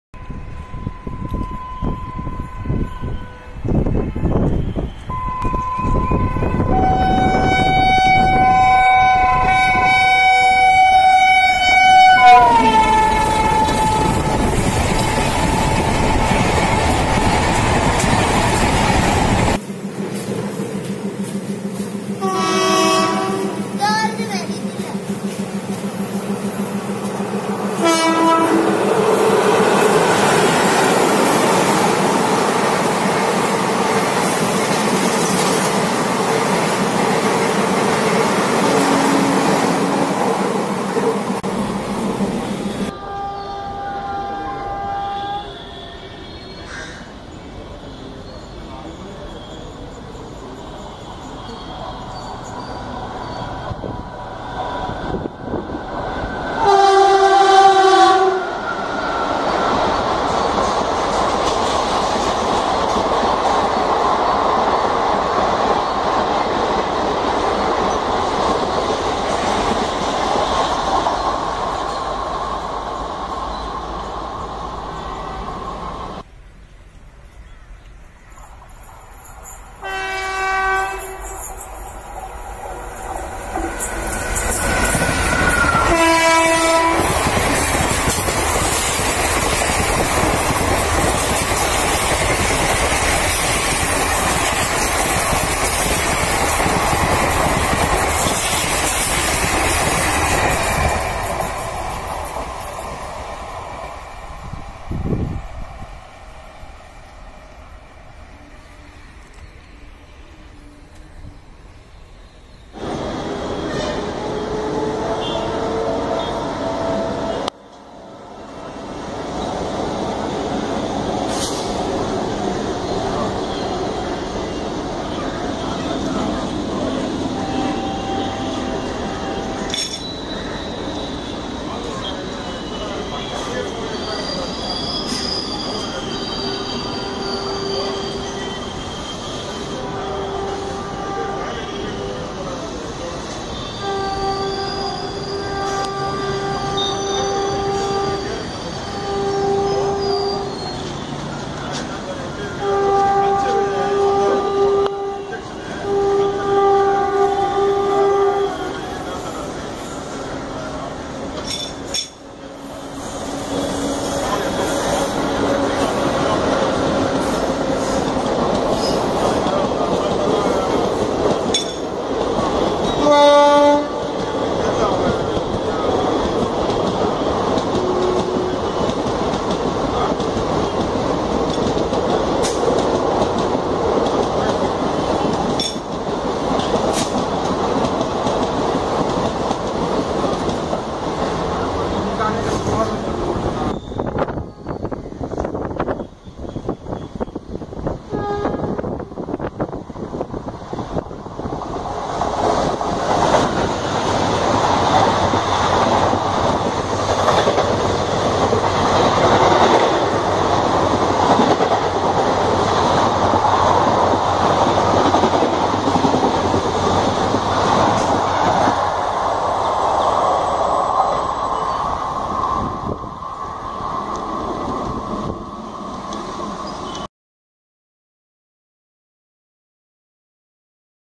300TH UPLOAD | LEGENDARY ARAKKONAM GRANDPA - AJJ WAP1 | POWERFUL LOCOMOTIVE | A SMALL COMPILATION
A very big thanks to all my dear viewers. This is my 300th upload and I'm very glad to upload this Keep supporting and once again thankyou all.

A small compilation of old Grandpa legendary AJJ WAP1 spotted in Chennai.

56037 Chennai egmore - Puducherry fast passenger
AJJ WAP1 #22008
Pallavaram

16101 Chennai egmore - Kollam(quilon mail) express
AJJ WAP1 #22053
Chrompet

17652 Kacheguda - chengalpattu express
AJJ WAP 1 #22031
Pallavaram

17652 Kacheguda - chengalpattu express
AJJ WAP1 #22015
Chrompet

17651 Chengalpattu - Kacheguda express
AJJ WAP1 #22015
Tirusulam